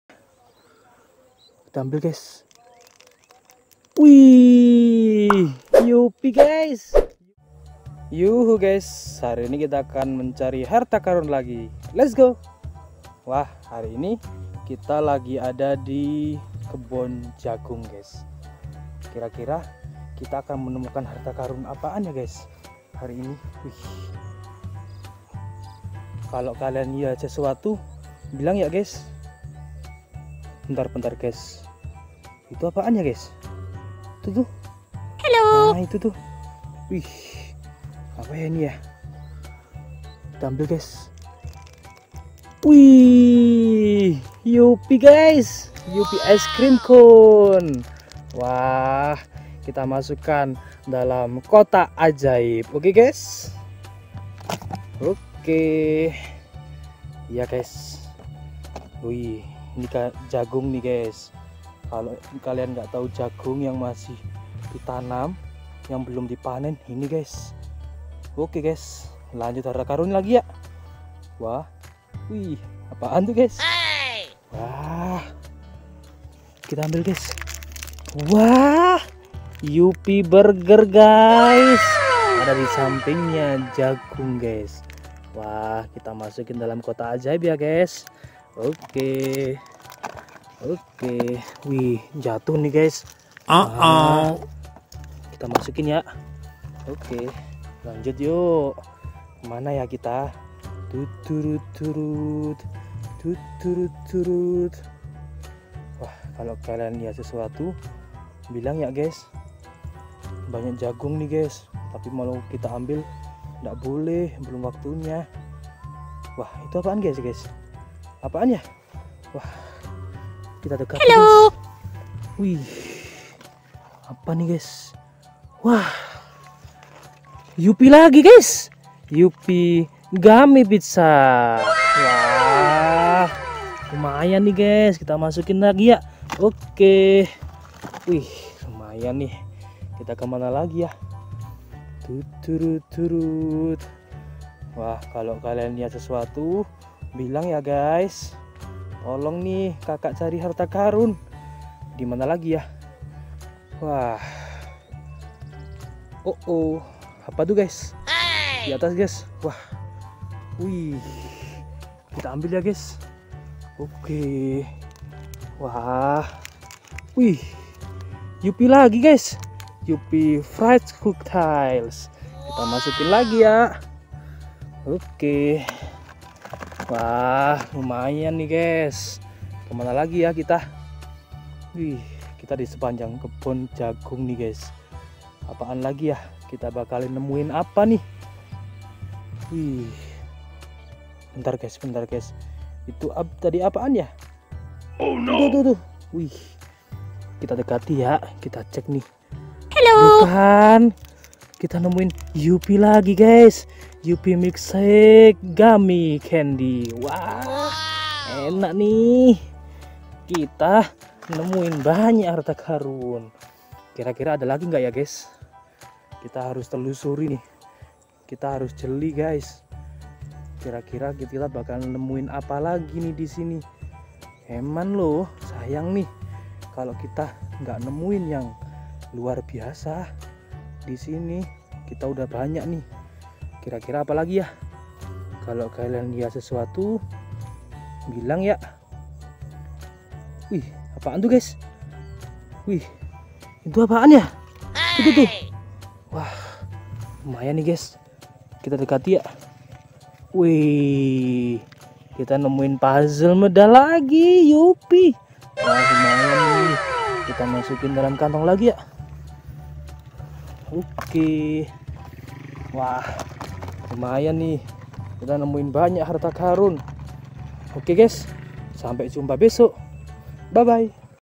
Mencari Macam Macam Permen Yupi, Yupi Burger, Gummy Pizza, Yupi Iced Cream Cone di Kebun Jagung
Just Fun!!

Let's Start Having Fun !!!